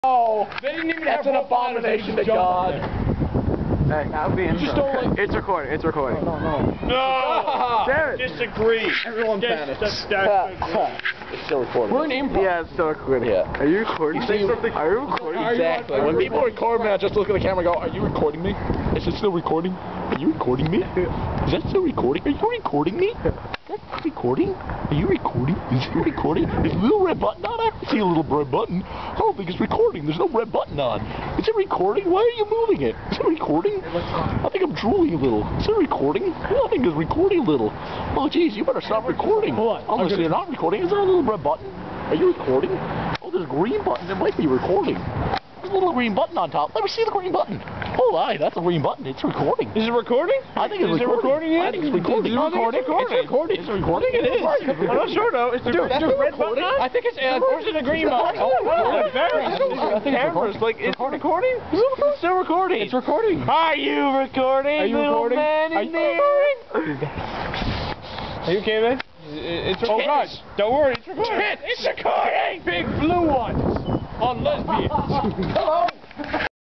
"Are you recording me?"
The five of us were waiting outside for our rides after improv.  I spoil the moment by taking out my camera, but start something fun.
I missed some excellent footage of Richard (the guy who starts it) singing about his daddy being late.